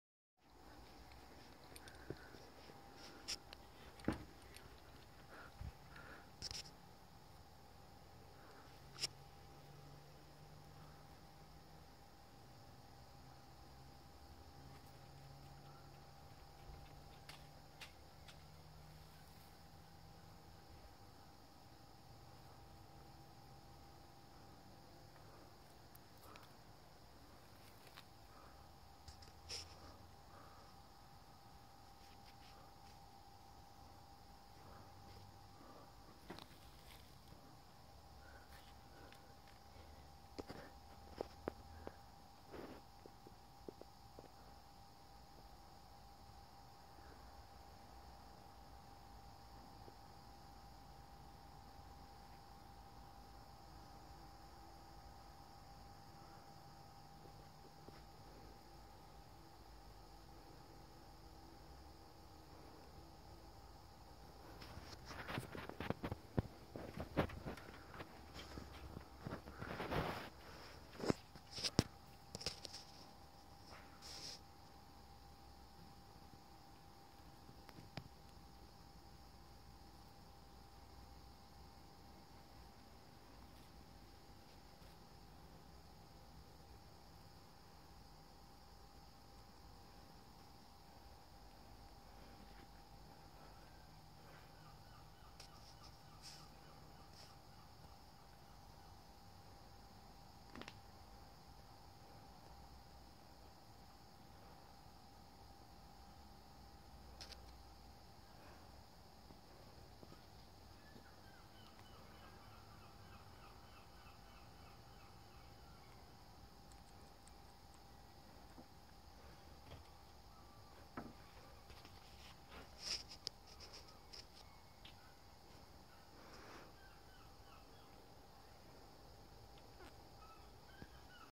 Mole in my muck pile
Found this mole munching on worms from my dung pile from the horses.